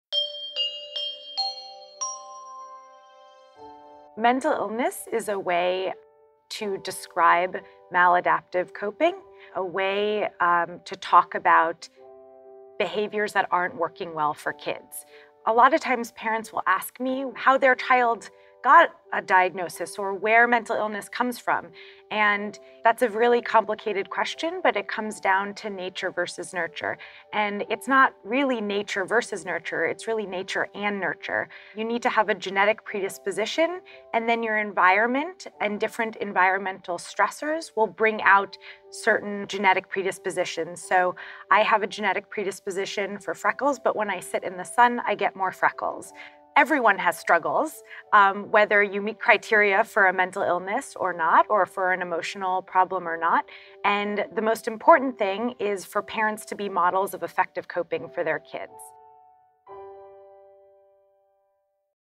Is Mental Illness Real in Kids?
Think of it as a form of coping, a set of behaviors that aren't working for a child. For more from the Child Mind Institute